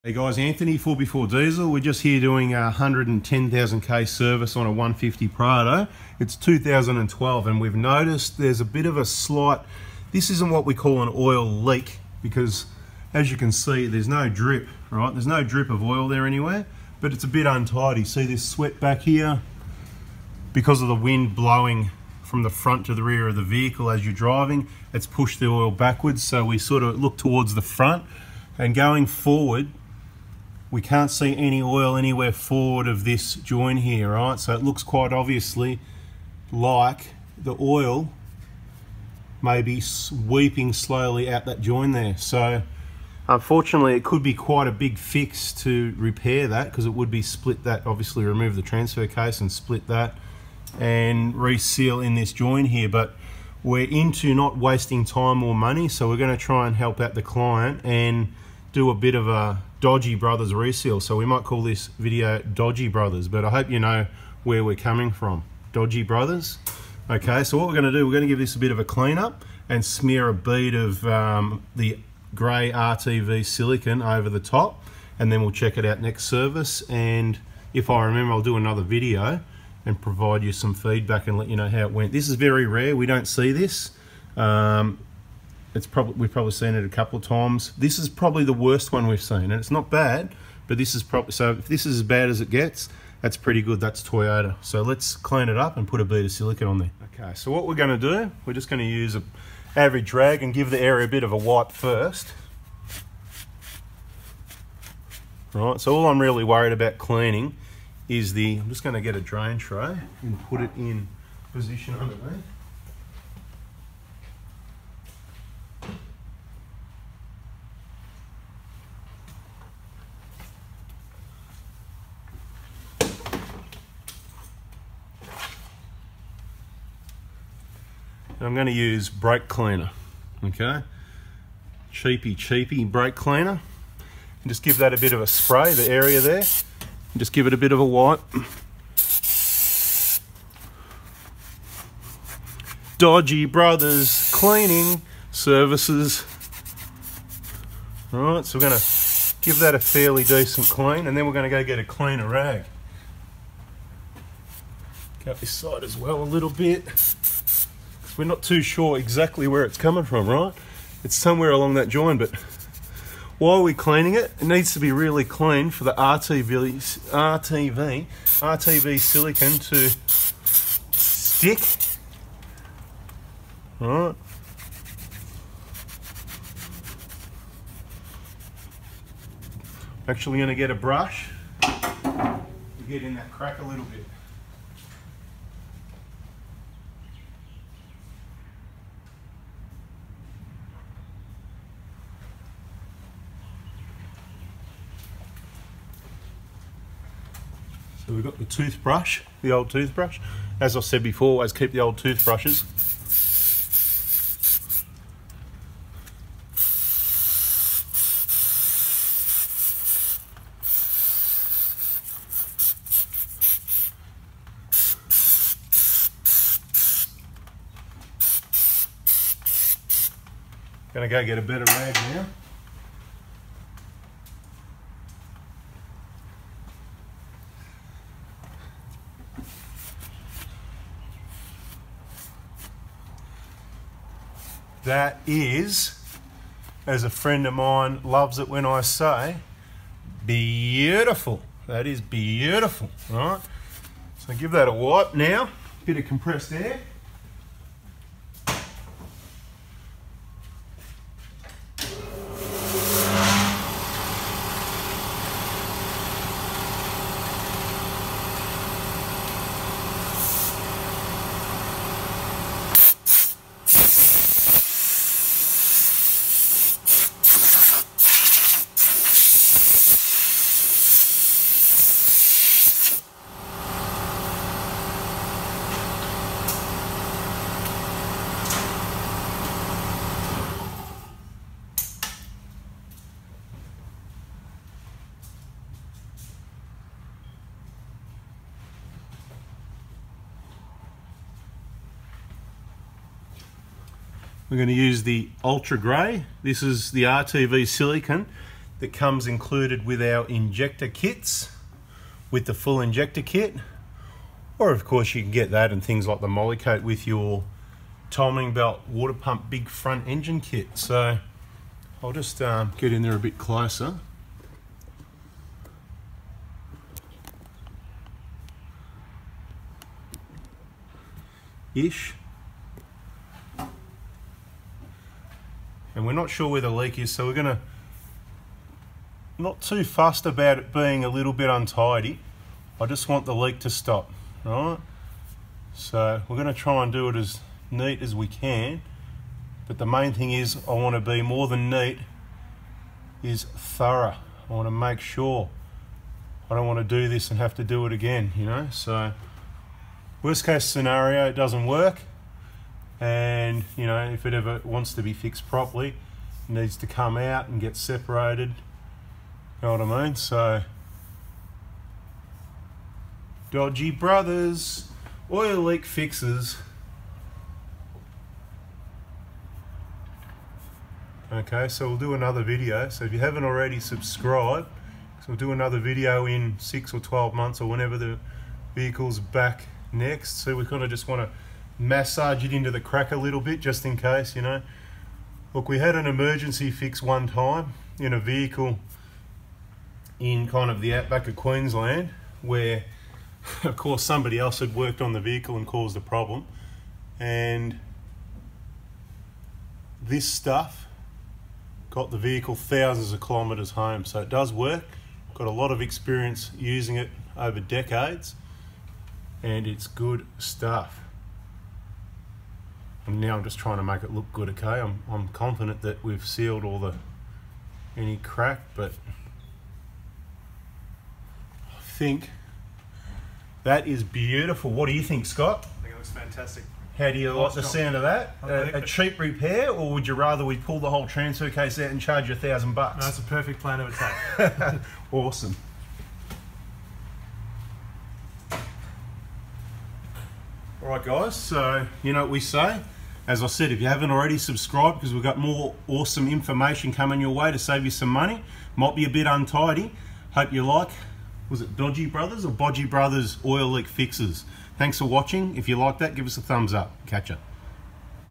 CHEAPEST OIL LEAK FIX THAT WORKS
Dodgy or Bodgy brothers? Please comment Bodgy, dodgy or smart. This is a Toyota Transfer case. Rarely or never see this on a Prado or Hilux. Nothing to worry about, little untidy but maybe better than the oil mess. How to fix an oil leak cheap. Avoiding unnecessary repairs for small issues.how to fix a transfer case and gearbox oil leak.